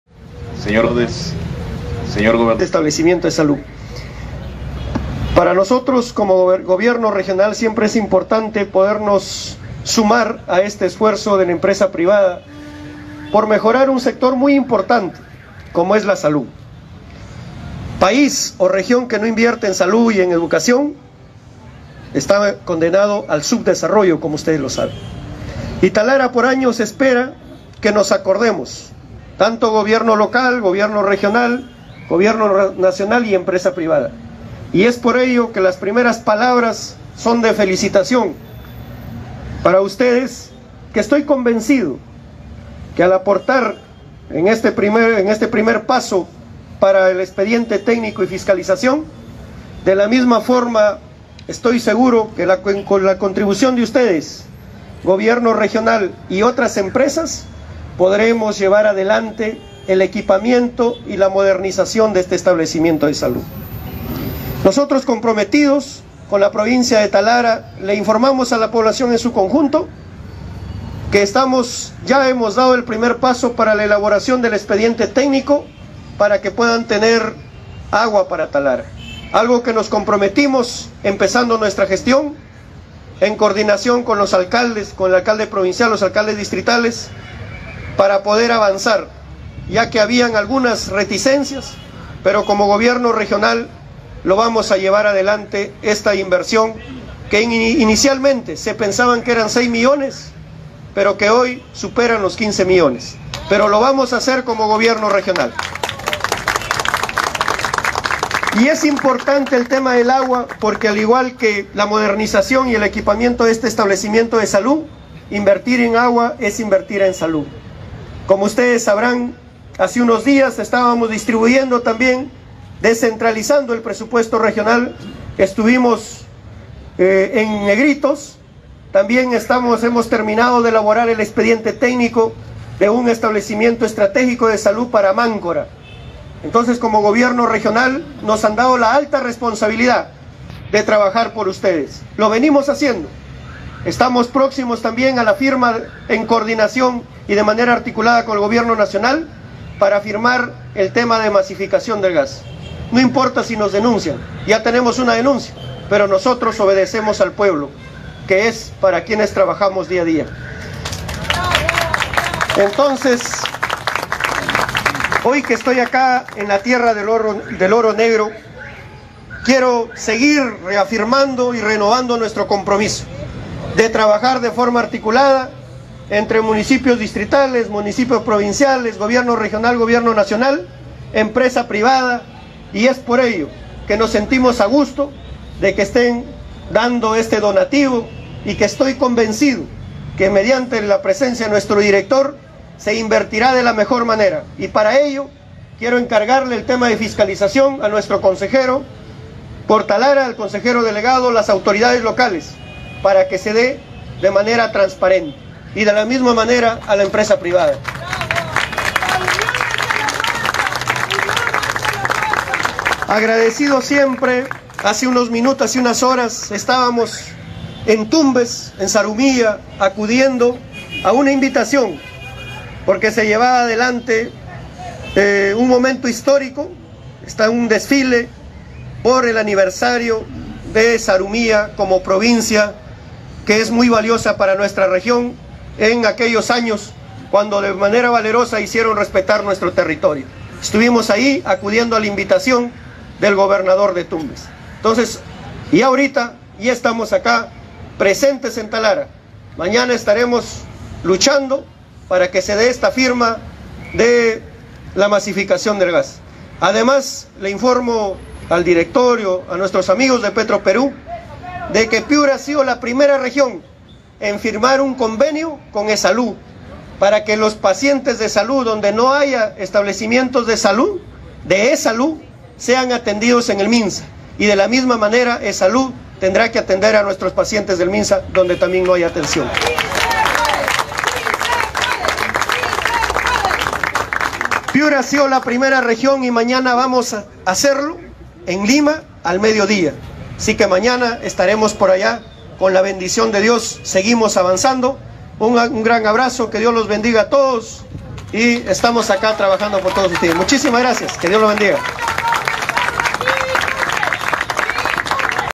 SERVANDO GARCÍA: "VAMOS A HACER EL EXPEDIENTE TÉCNICO PARA EL EJE PAITA-TALARA"
En su visita a Talara para recibir la donación económica de petroperú para el hospital Minsa Talara II, el Gobernador Regional también señaló que el expediente técnico para le eje Paita-Talara que inicialmente costaba 6 millones, costará algo mas de 15 millones pero el GORE Piura lo hará, así mismo espera que antes del 28 de Julio se firme la masificación de gas en la Región "No importa que me denuncien, nos debemos al pueblo", y que Minsa ha suscrito un convenio con Essalud de cooperación mutua.